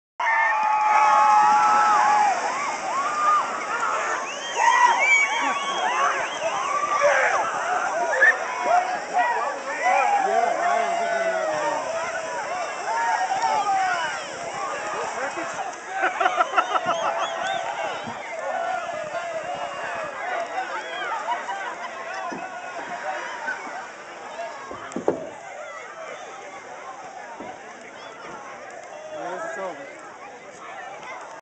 Polar Bear Plunge Lake George NY New Years Day 2011
This is the 3rd and LAST Polar Bear Plunge 3pm Saturday January 1st 2011